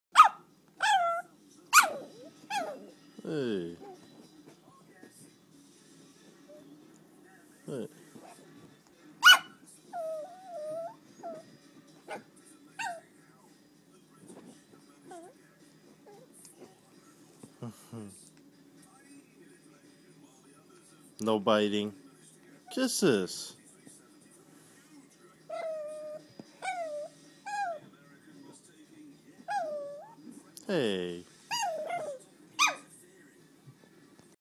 Bug crying to get on chair
Bug's cute cry trying to make me feel bad so she get on the chair with me